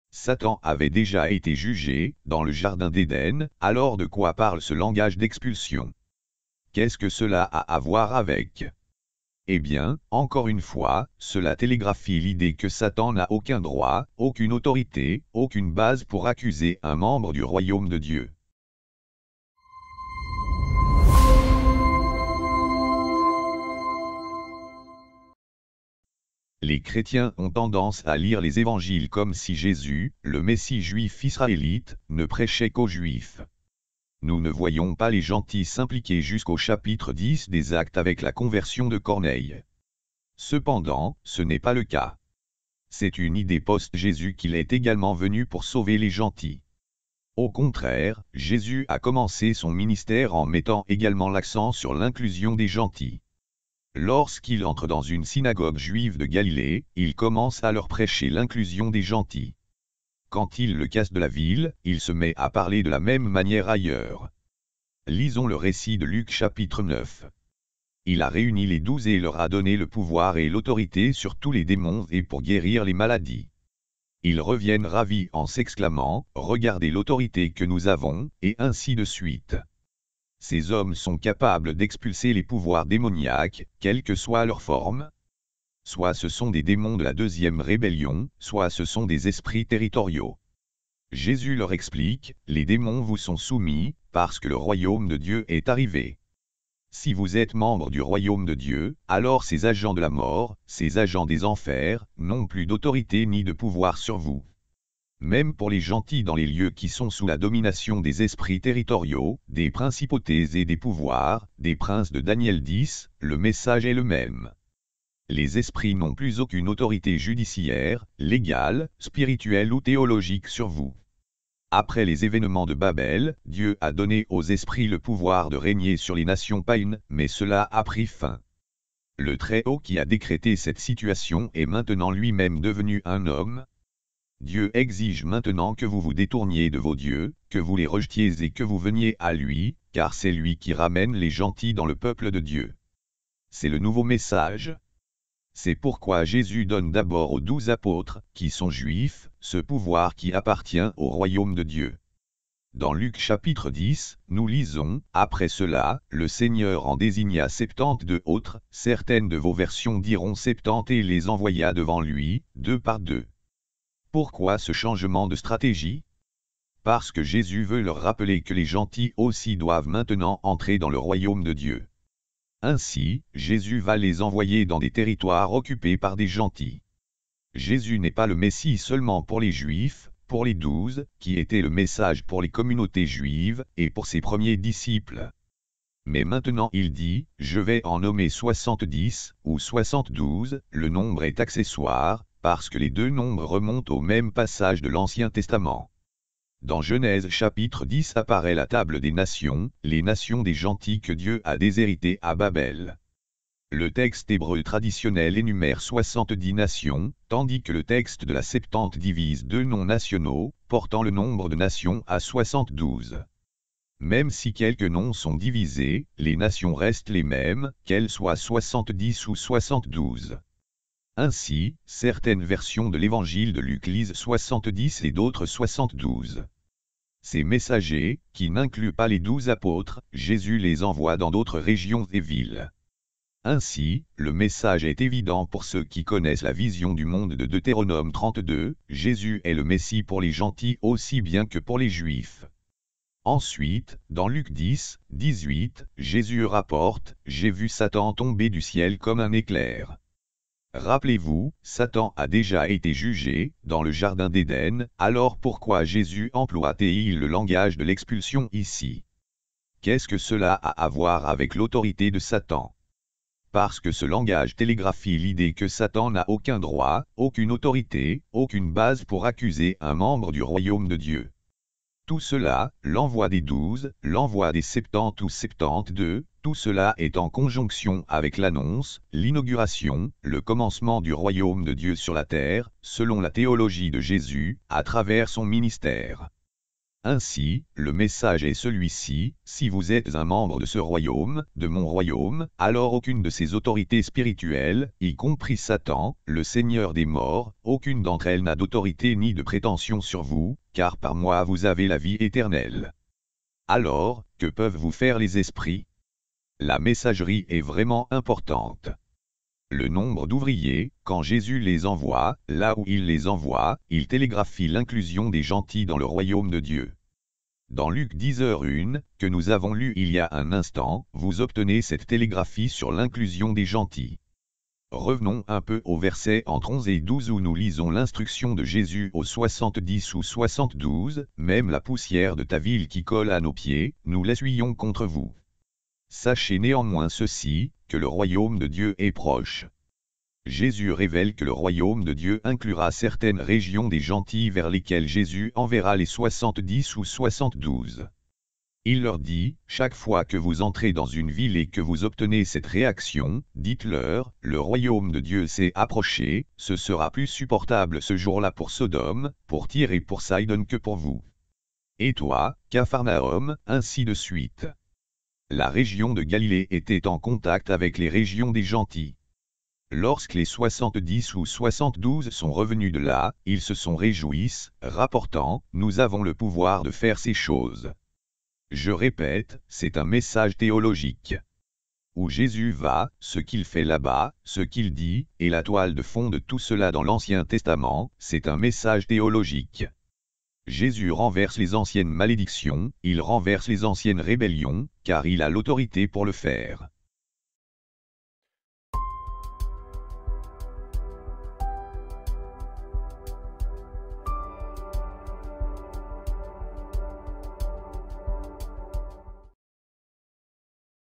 Comment Jésus a montré qu'il était aussi le Messie des Gentils
Dr Michael S Heiser

Les chrétiens ont tendance à lire les évangiles comme si Jésus, le Messie juif israélite, ne prêchait qu'aux juifs. Nous ne voyons pas les Gentils s'impliquer jusqu'au chapitre 10 des Actes avec la conversion de Corneille. Cependant, ce n'est pas le cas. C'est une idée post-Jésus qu'il est également venu pour sauver les Gentils. Au contraire, Jésus a commencé son ministère en mettant également l'accent sur l'inclusion des Gentils. Lorsqu'il entre dans une synagogue juive de Galilée, il commence à leur prêcher l'inclusion des Gentils.

Adapté de "Comment Jésus a montré qu'il était aussi le Messie des Gentils" du 19 septembre 2022